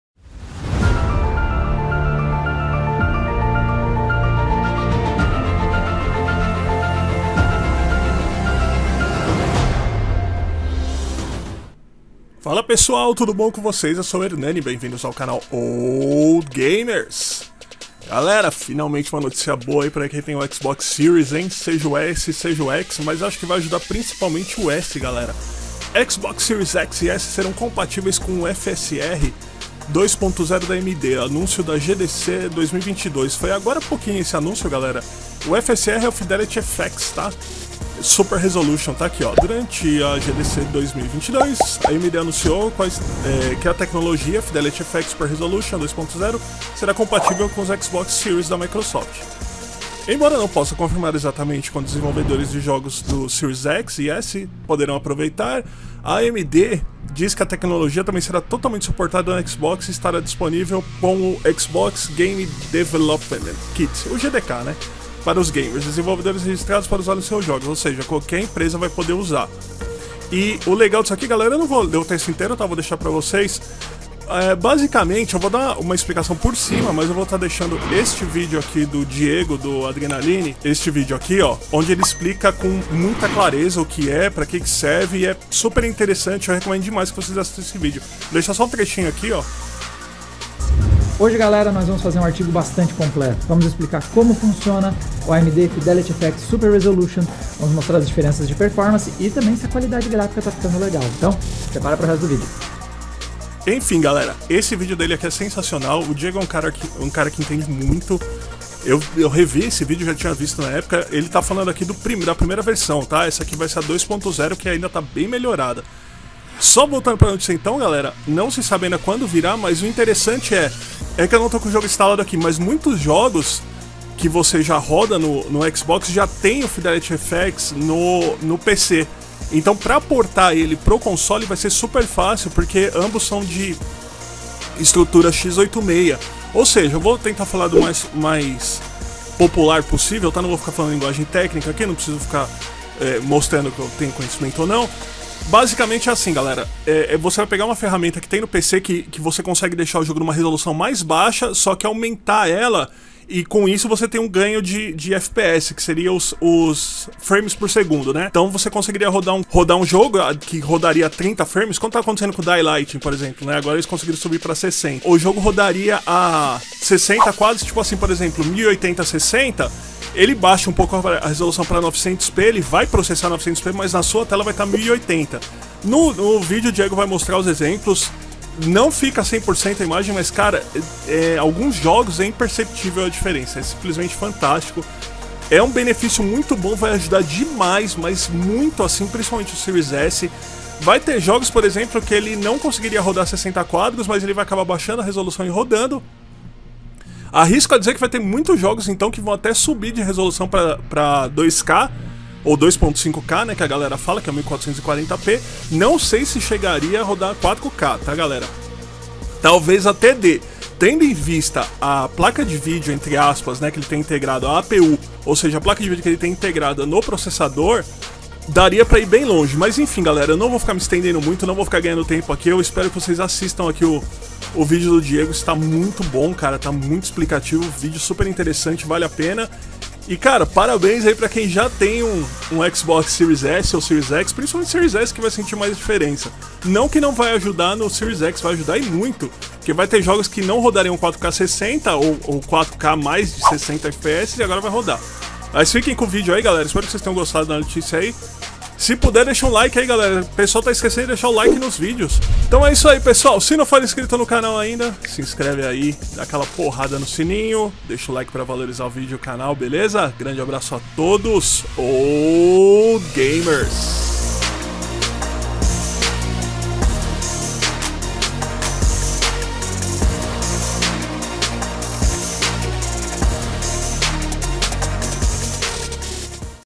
[NEWS!] Quais as melhorias traz ao Xbox Series X e S?
Neste vídeo comento sobre esta incrível melhoria que chegará em breve aos consoles de nova geração Xbox, e quais melhorias poderemos usufruir?

Se você curte o conteúdo do canal e deseja ajudar com qualquer valor:
O canal ainda não é monetizado, então todo incentivo é bem vindo :)